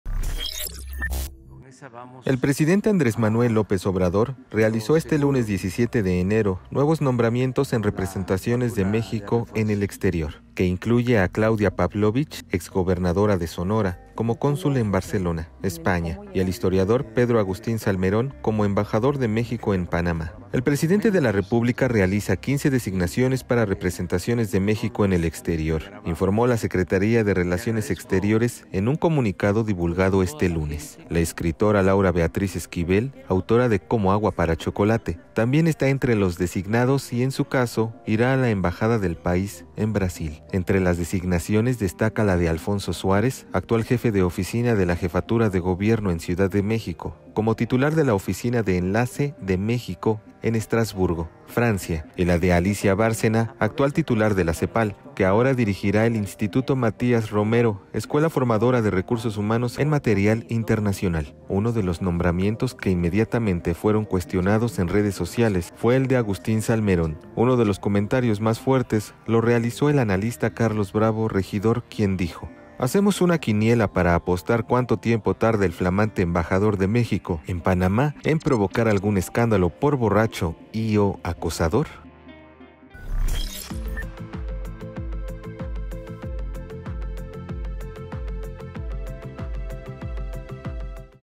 AMLO designa a Claudia Pavlovich cónsul en Barcelona y a Pedro Salmerón a la embajada en Panamá
El presidente realizó 15 nombramientos para diferentes representaciones de México en el exterior que incluye a Alfonso Suárez, actual jefe de oficina de la Jefatura de Gobierno en CDMX, como titular en la Oficina de Enlace de México en Estrasburgo y otros